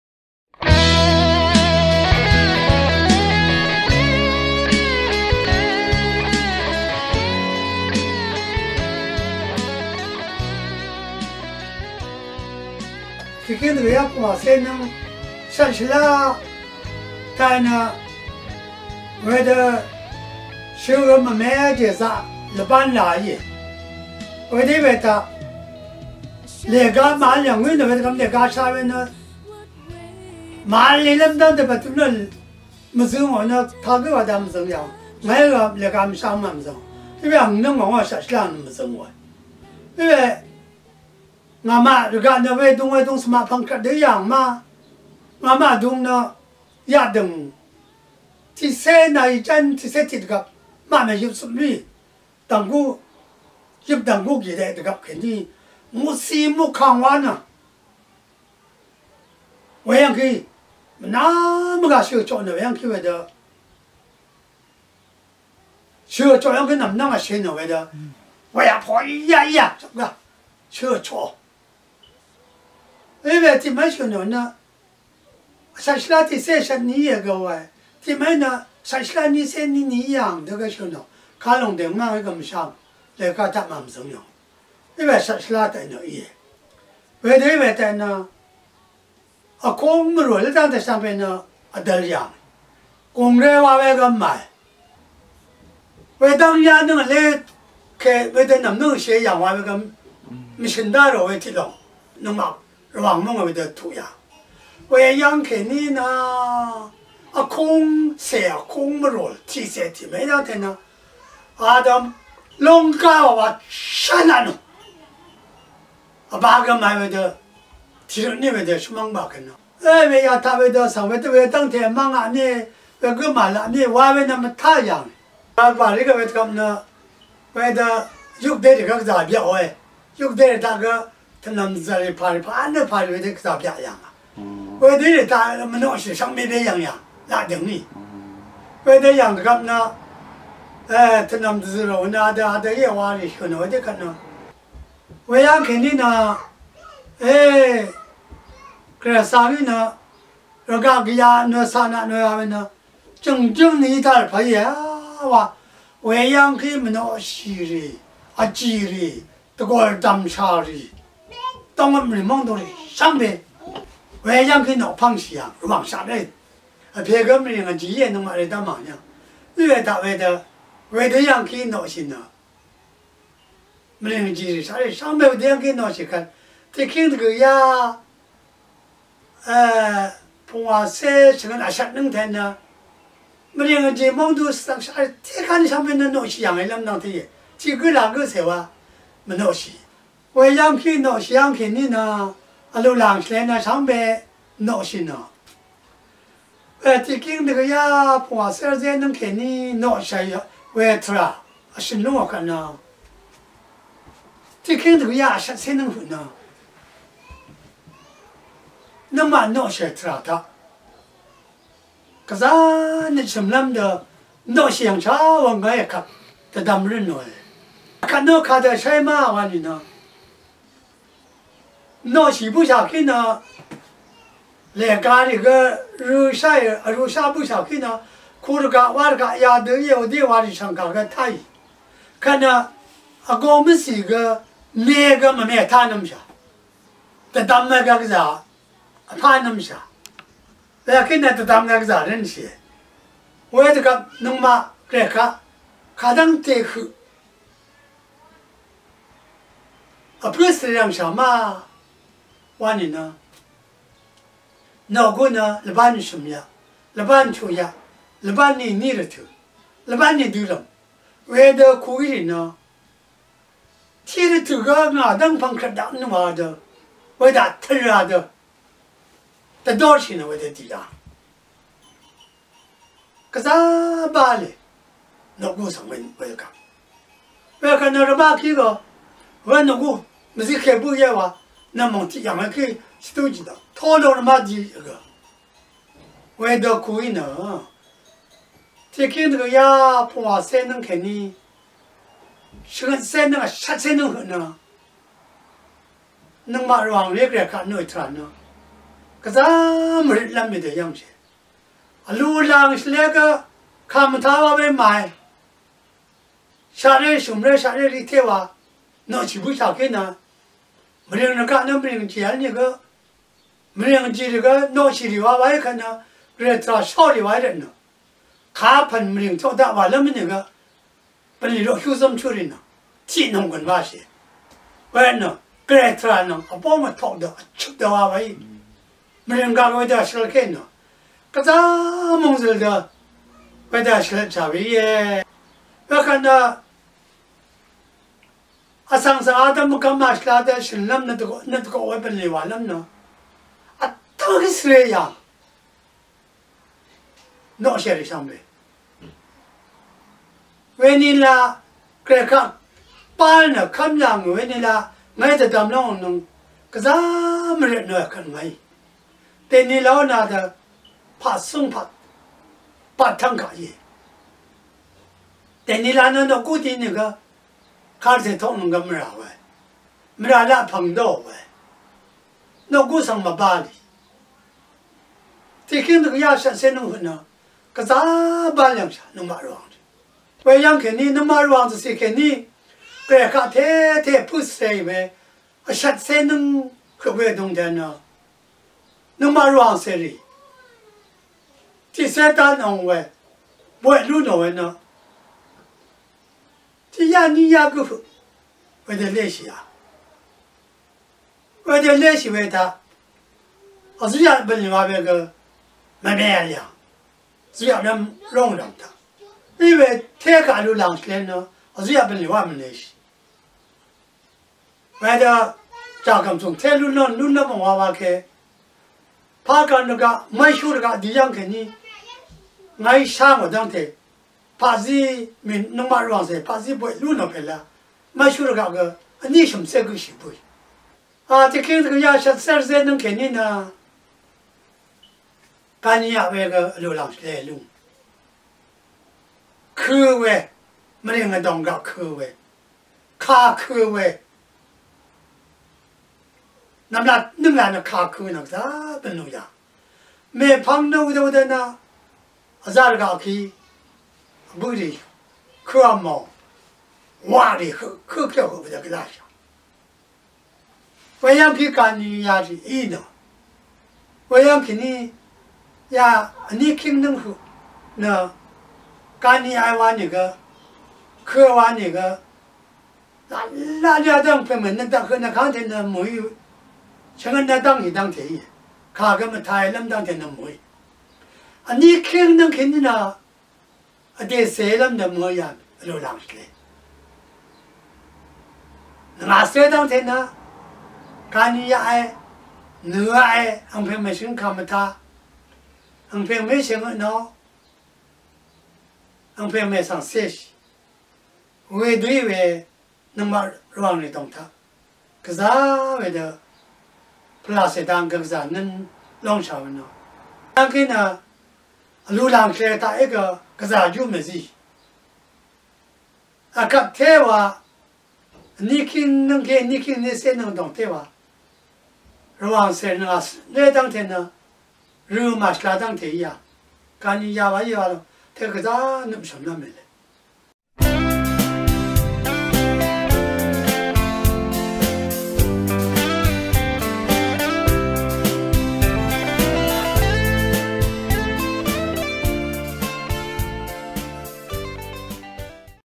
Rvwang History (Nvmlat noqshì ráshà àngni dvjà)
1) Rvwàngrì noqshì lv́m Gvrayí nvmnǿ dvzø̀l yàngà dang
2) Nvmlat noqshì nila Rvwangrìò Kv́mnøt dang
3) Kv́mnøt zidang vjá pv́ngshì ráshà dang
Ang vdè neí yàngò nø àng vdèí vhǿm yàngà tvrà shø̀nò lòng ta shàì. Vpǿ vtvng mvshǿl íe. Matashì mèwani.